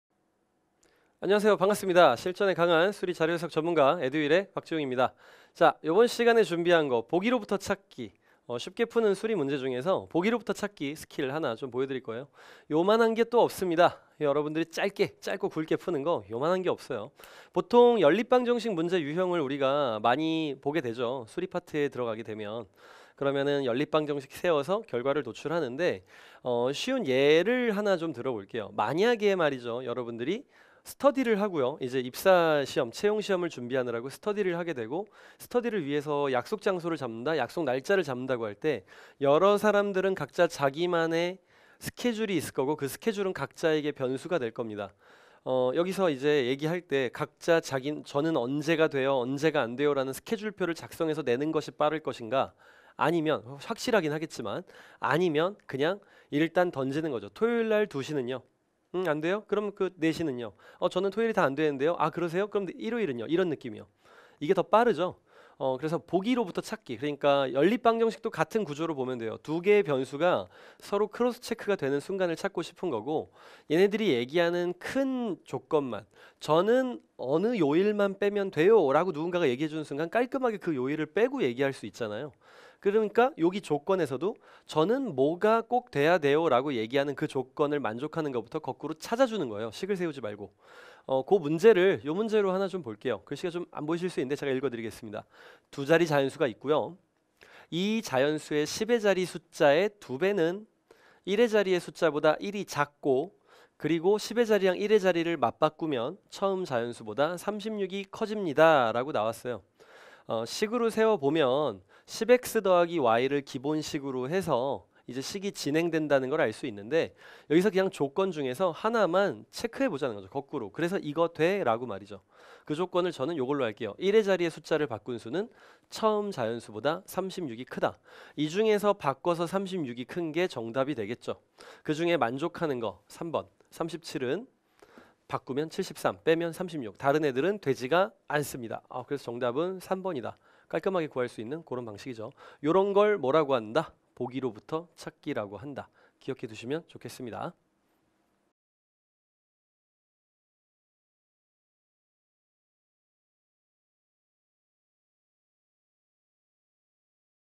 ★수포자 필수시청★ 수리능력 시간단축 스킬 大공개! ['보기'로부터 찾기 편] with. 박지웅 교수님｜에듀윌 취업
수리·자료해석 전문가 박지웅 교수님의
매주 1개 수리능력 풀이스킬 정복하기!